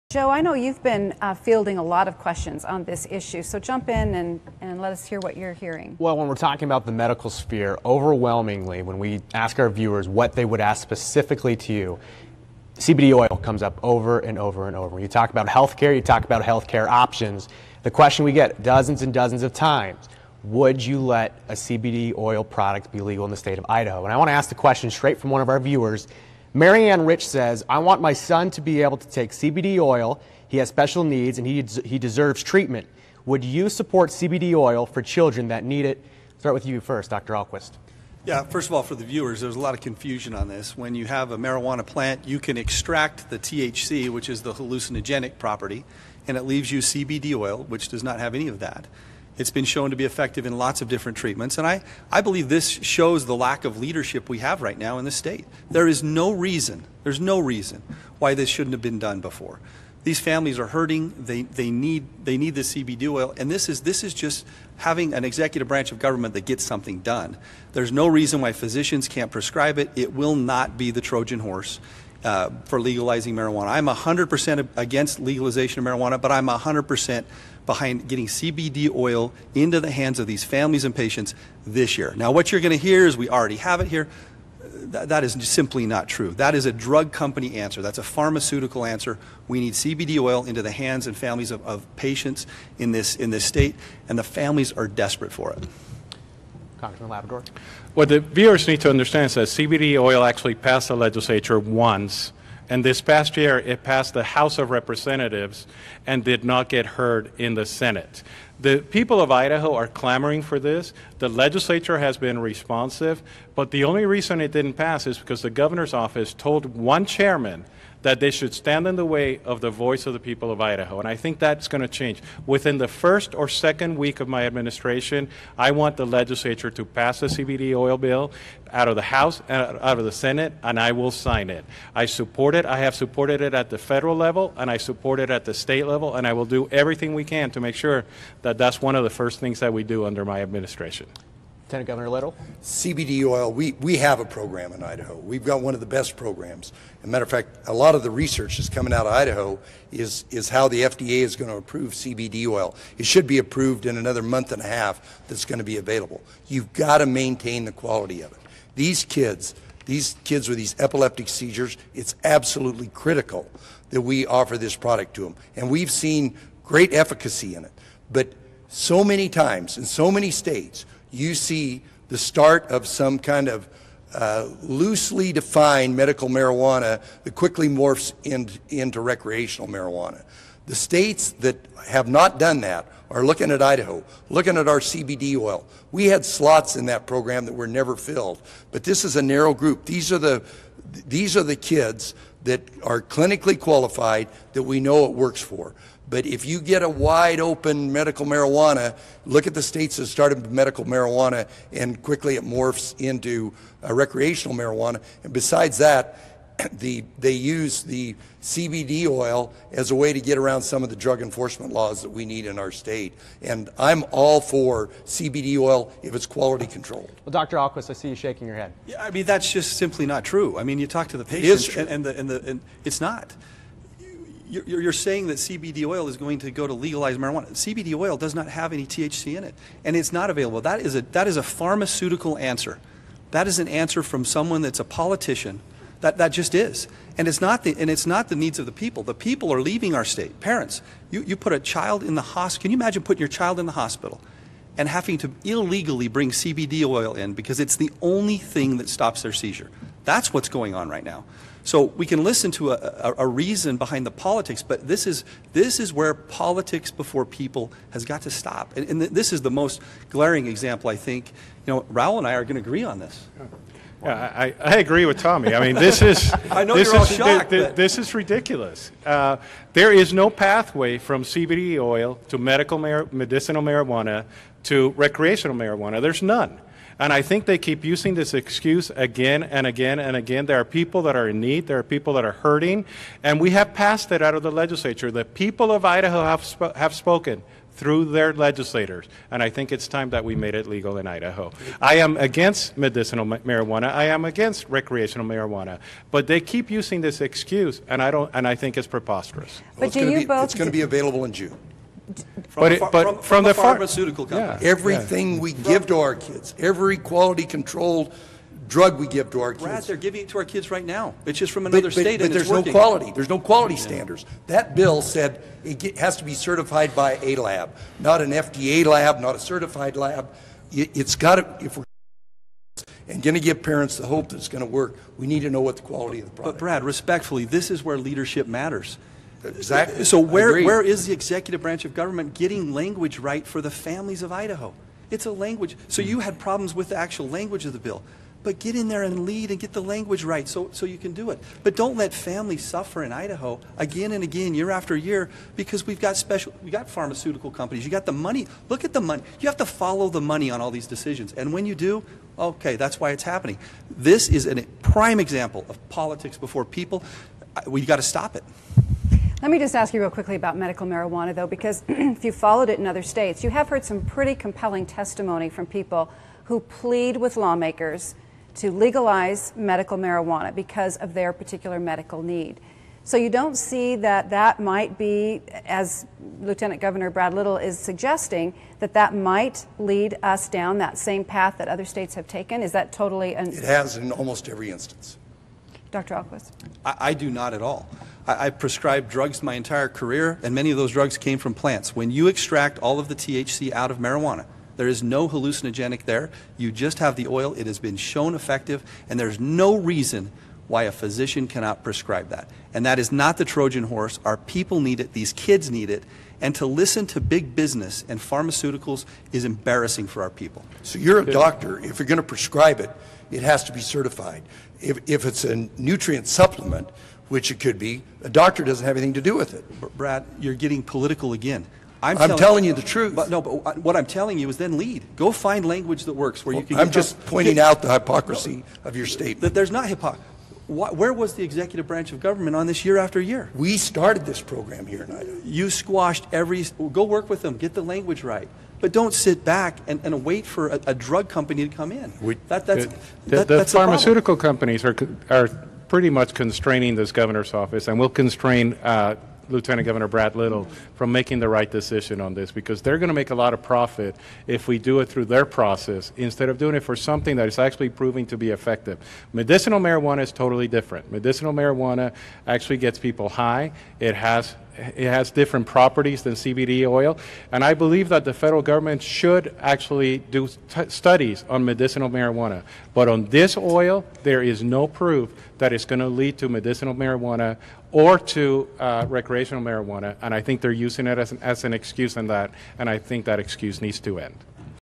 Idaho's Republican candidates for governor spar over CBD oil legalization
Video credit: KTVB 7
Debate date: April 30, 2018